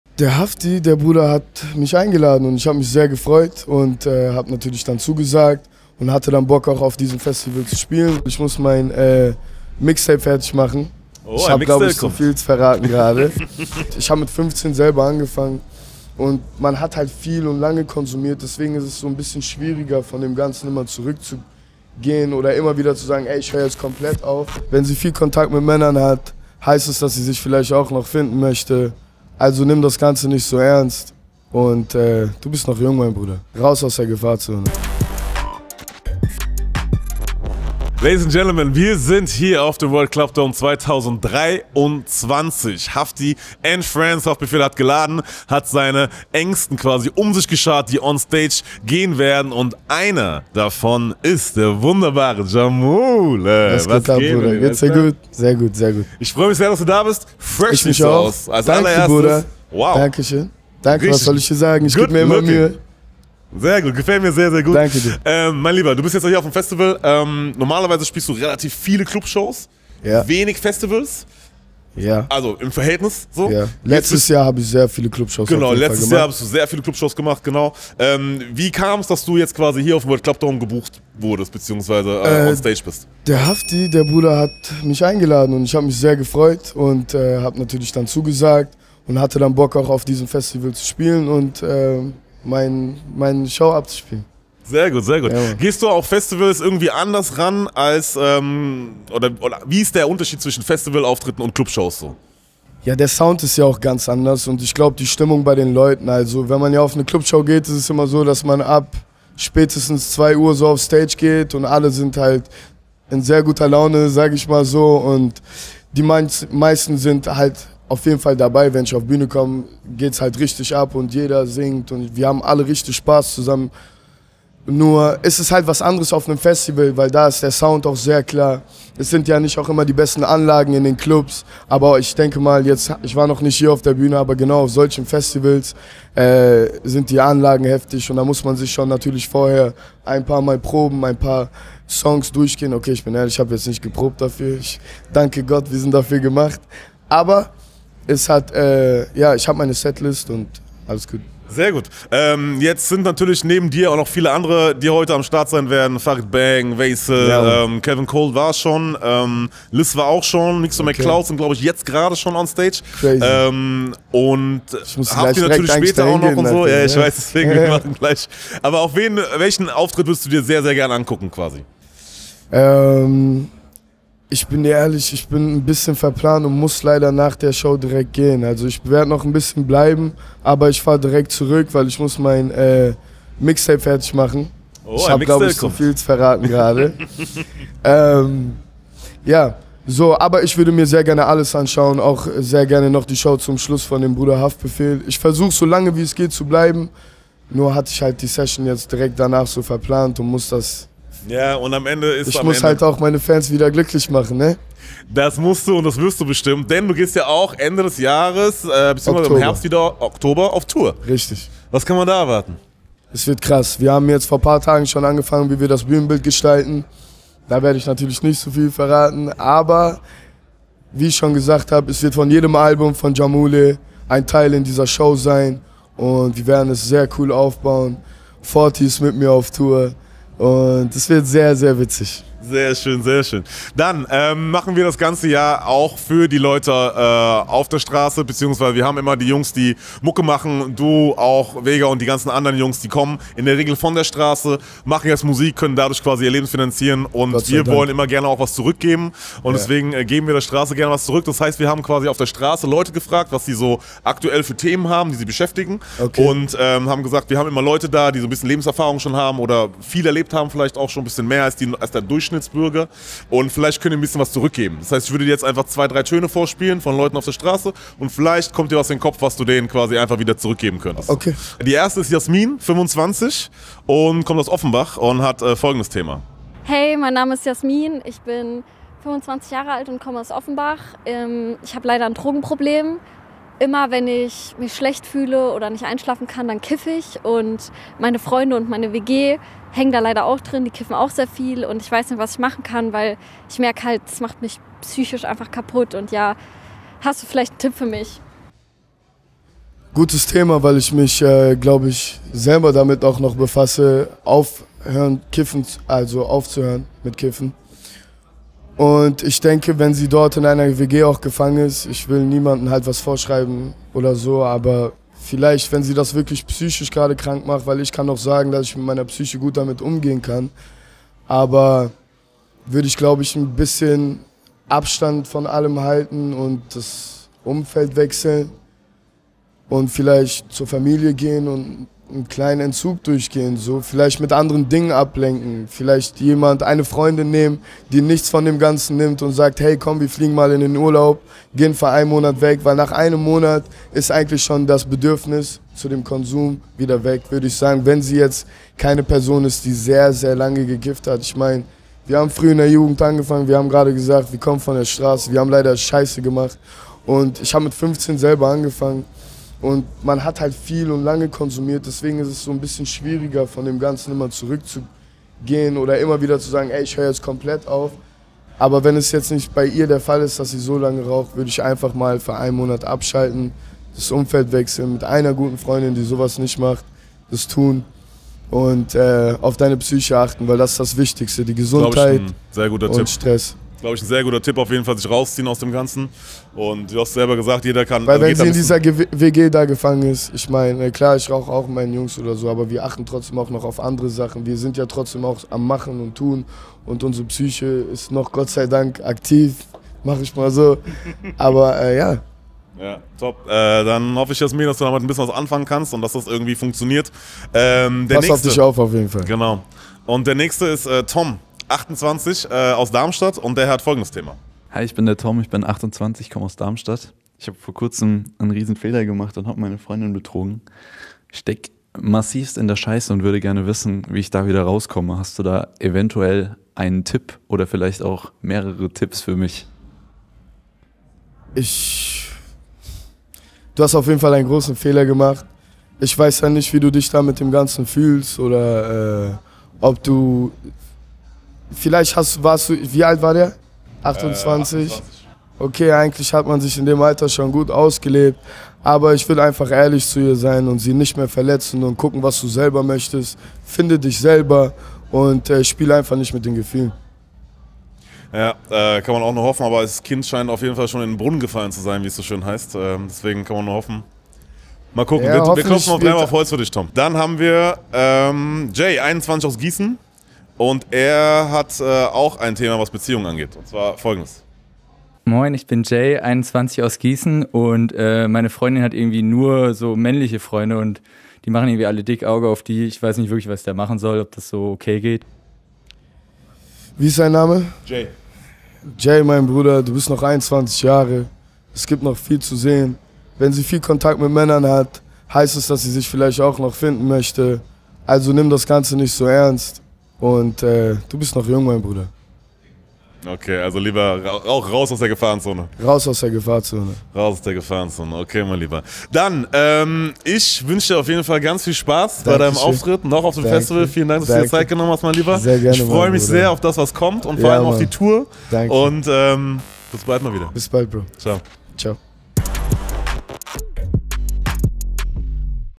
Jamule über seine Psyche, Umgang mit Drogen, geplantes Mixtape, Chaos-Tour ft. Fourty | Interview
Simon hat Jamule nach seinem Auftritt auf dem World Club Dome 2023 getroffen. Im Interview verrät Jamule, wer auf seiner bevorstehenden Tour dabei sein wird und gibt Details zu seiner Setlist preis. Dabei droppt er, dass wir uns dazu noch auf ein kommendes Mixtape freuen dürfen. Es wird auch emotional: Jamule zeigt sich von seiner verletzlichen Seite. Er spricht offen darüber, wie schwer es ist, den Drogen abzuschwören. Warum es mehr als wichtig ist, die eigene Psyche zu schützen und nicht mit den Gefühlen anderer zu spielen, erfahrt ihr ebenfalls.

Timecodes:
00:00 Highlights
00:34 Intro
01:05 Eindrücke vom WCD
02:51 Mixtape kommt!
03:25 Exklusive News zur Tour
03:53 Drogenproblem: “Achte auf deine Psyche”
06:49 “Spiele nicht mit den Gefühlen von Frauen”
07:47 Wenn sie viel Kontakt mit Männern hat
08:29 Outro

► YOU FM ist das junge Programm des Hessischen Rundfunks im Radio &

►Credits:
Redaktion: Vanessa Völker, Vivien Jaschok, Cedric Dilling
Moderation: Simon Vogt
Kamera: Flo See
Schnitt: Ludwig Stollenwerk & Kora Bender